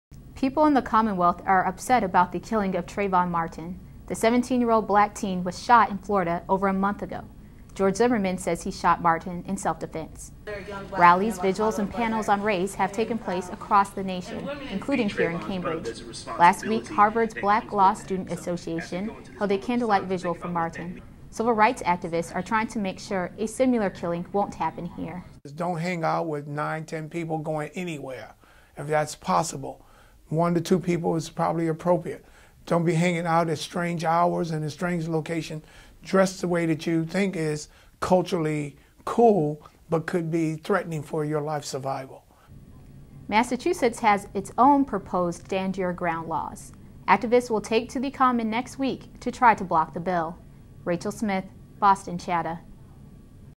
Trayvon Martin
Urban League of Eastern Massachusetts President Darnell Williams gives advice on how to prevent a future Trayvon Martin killing in Boston.